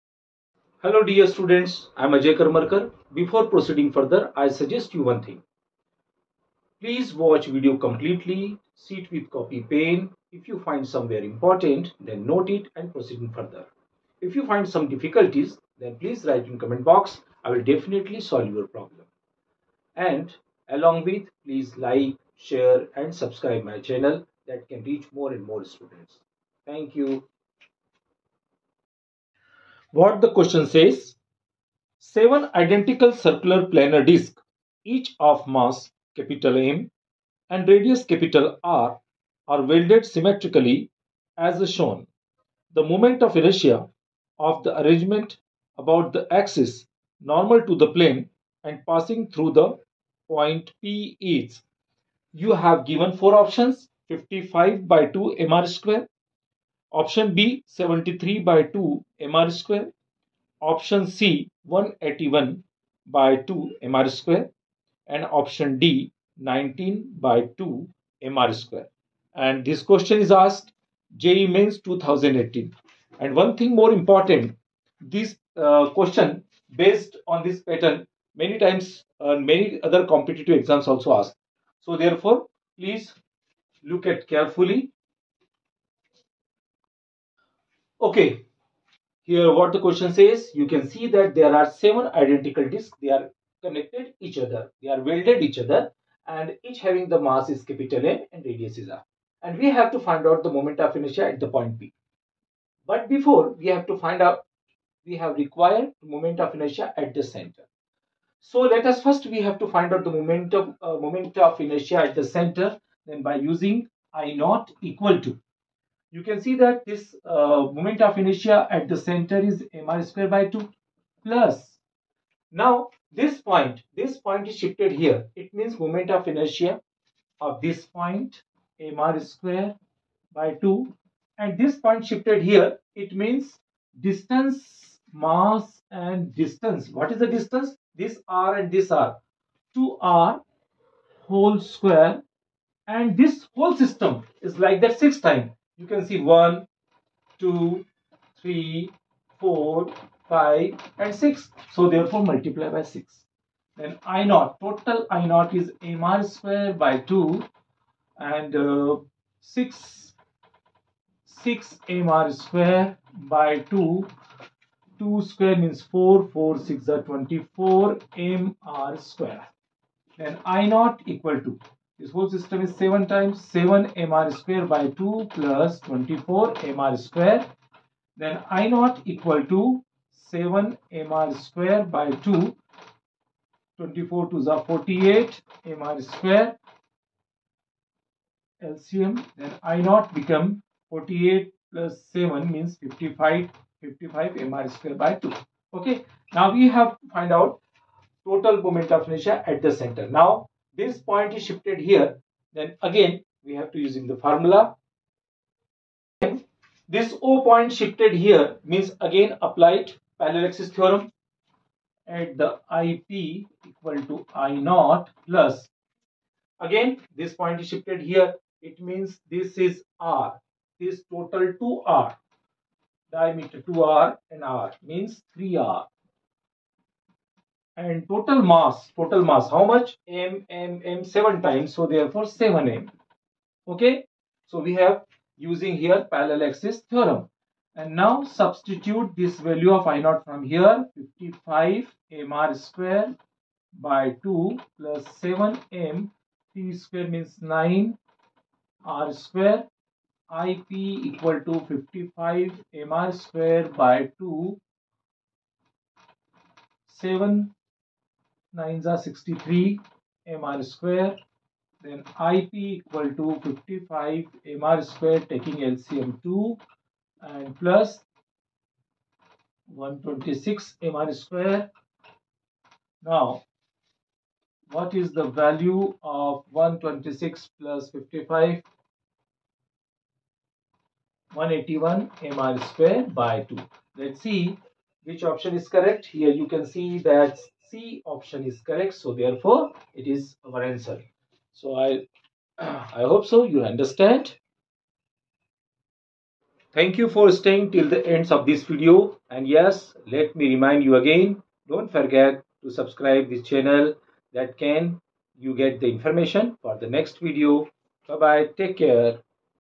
seven identical circular planar discs each of mass M and radius R are welded symmetrically as shown.
Seven identical circular planar discs, each of mass M and radius R welded symmetrically as shown.The moment of inertia of the arrangement about the axis normal to the plane and passing through the point P is